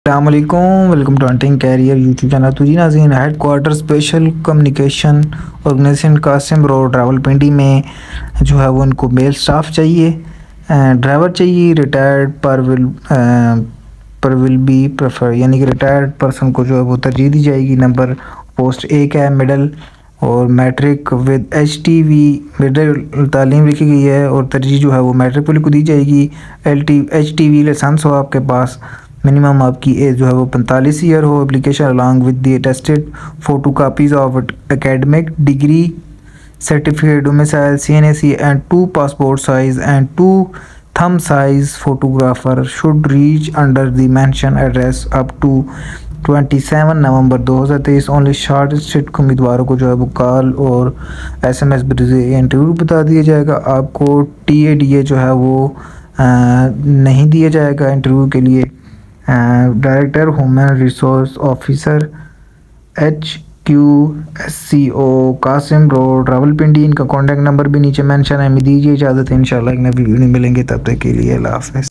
HEADQUARTERS SPECIAL COMMUNICATIONS ORGANIZATION JOBS 2023 | HSCO jobs 2023
Welcome to the latest job opportunities at HEADQUARTERS SPECIAL COMMUNICATIONS ORGANIZATION (HSCO) for the year 2023! In this video, we'll explore the various job openings, including SSCO jobs 2023. Additionally, we'll discuss the growing trend of work-from-home jobs in 2023, providing you with information on the best opportunities and ways to secure remote jobs.